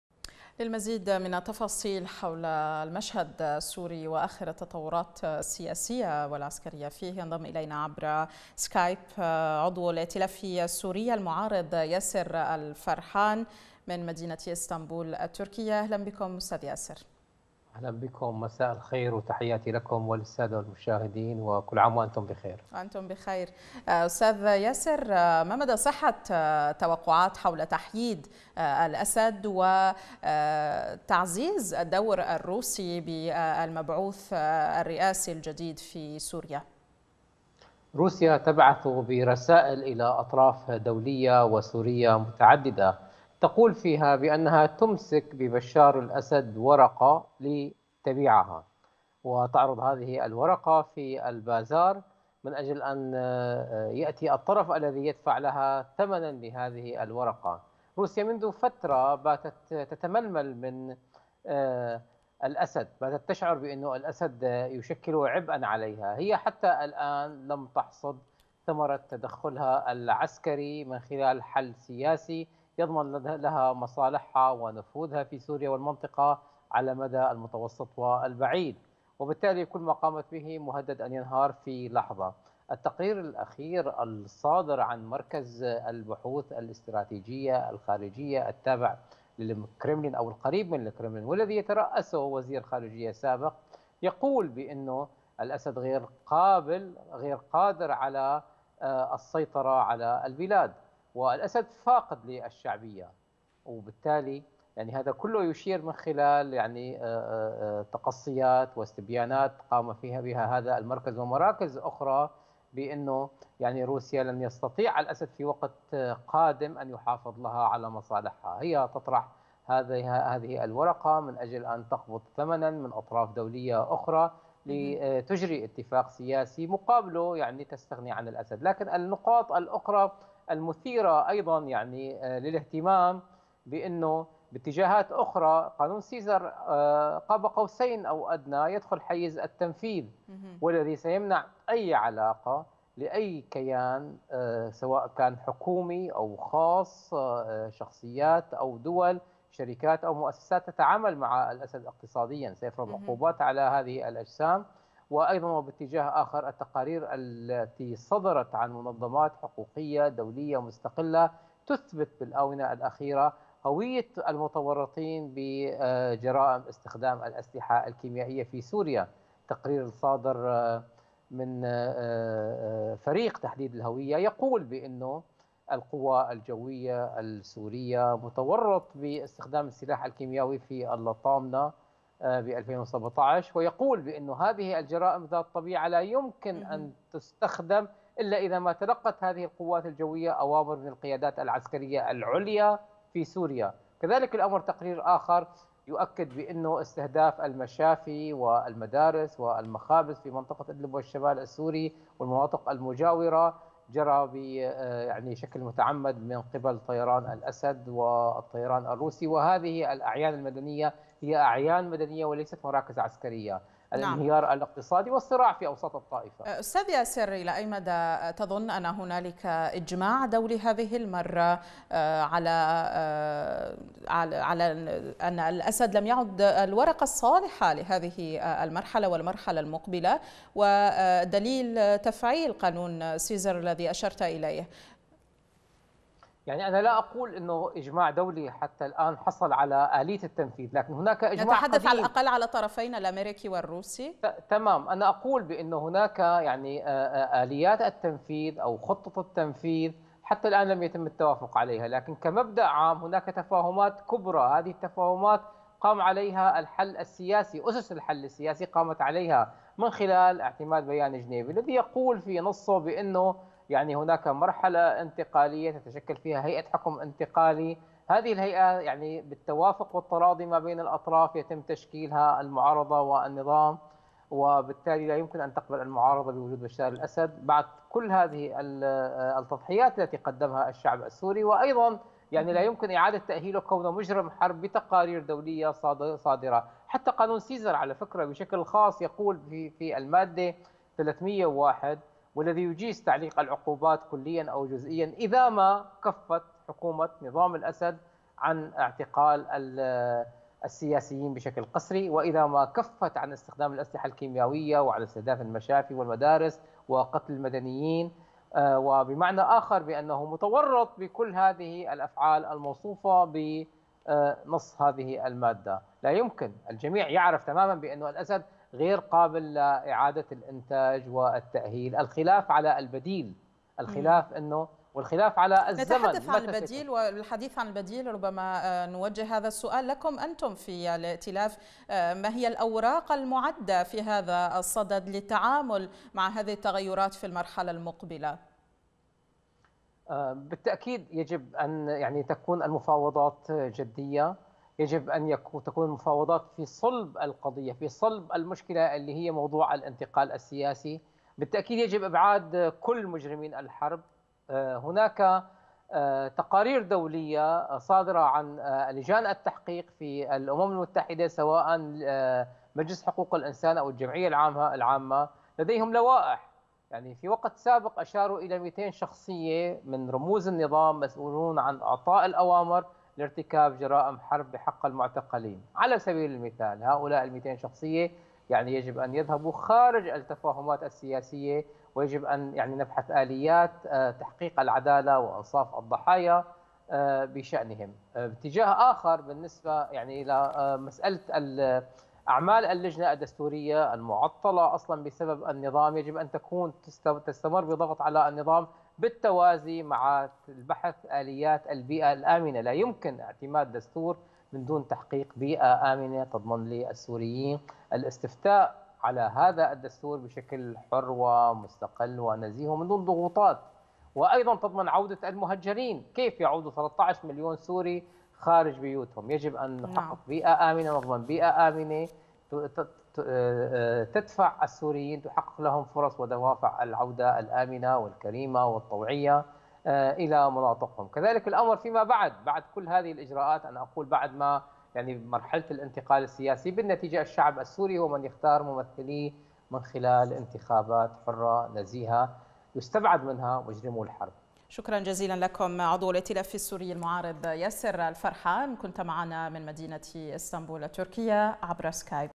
مداخلة ياسر الفرحان بشأن آخر تطورات المشهد السوري في ظل الخلافات التي تعصف بنظام الأسد
ياسر الفرحان: المعارضة السورية لن تقبل ببقاء بشار الأسد بعد التضحيات الكبيرة التي قدمها الشعب السوري طوال الفترة الماضية
ــــــــــــــــــــــــــــــــ